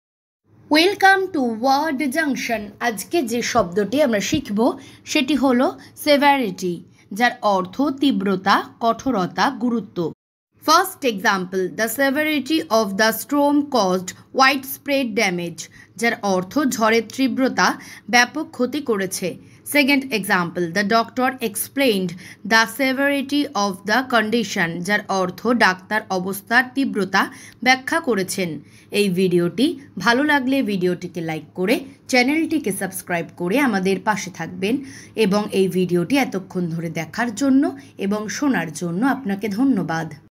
Severity Meaning in Bengali/Severity Mane Ki, Severity Explain in Bengali
Hi Friend this Video is about Severity Meaning in Bengali/Severity Mane Ki
Severity Explain in Bengali